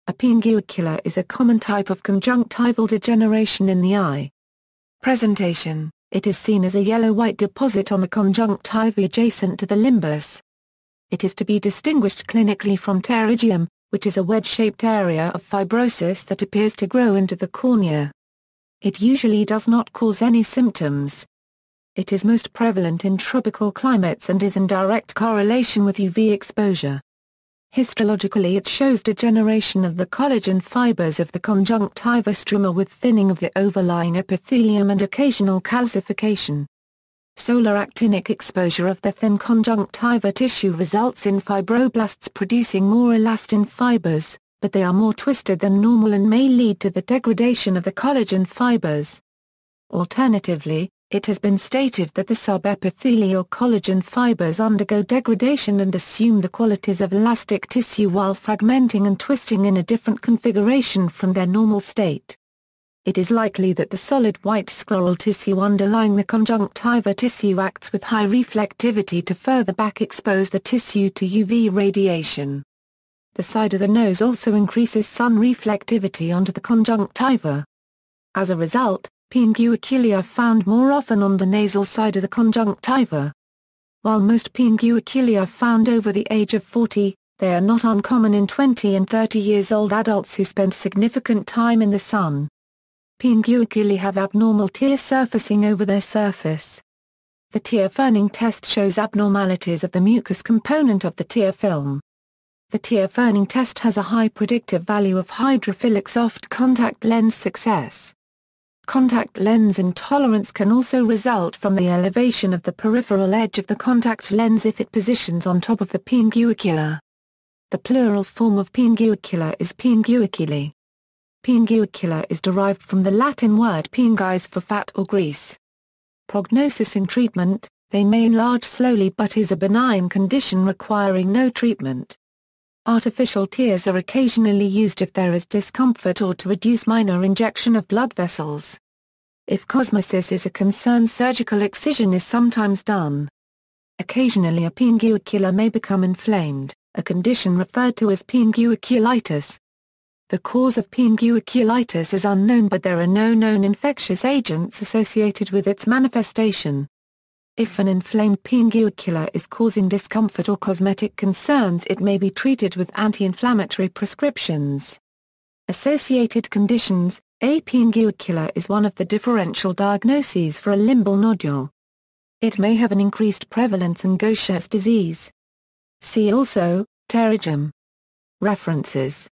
Pinguecula
A pinguecula is a common type of conjunctival degeneration in the eye.